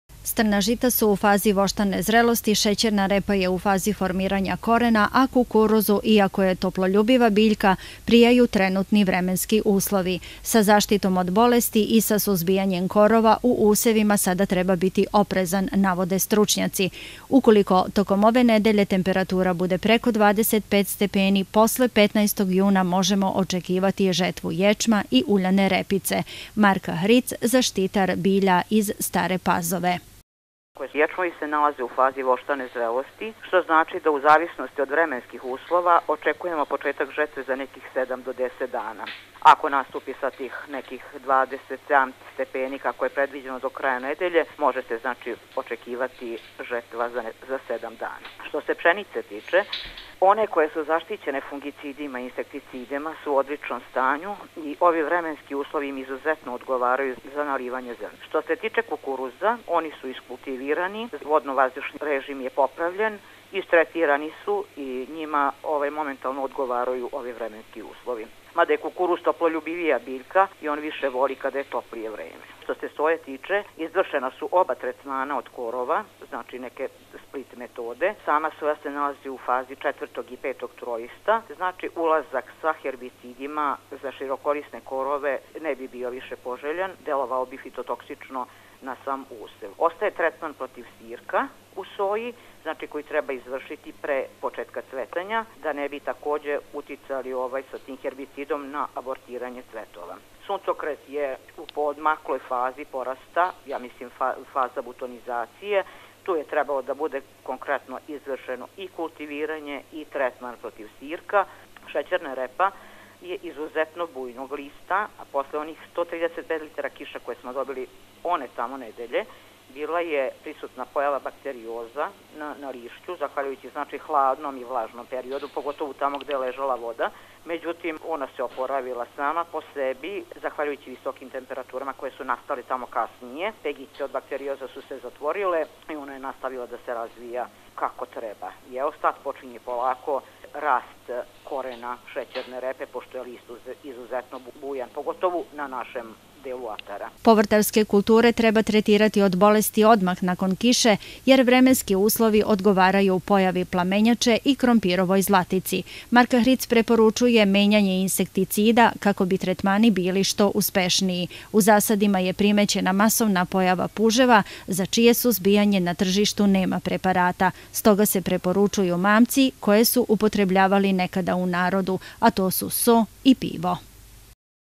Zaštita useva i stanje istih
Tokom vikenda na području staropazovačke opštine zabeleženo je 4,5 litre  kiše po kvadratnom  metru. Od  padavina slabog intenziteta pokorica  zemlje nije razbijena, kažu agrometeorolozi, ali dodaju, da je stanje useva u ovom trenutku zadovoljavajuće.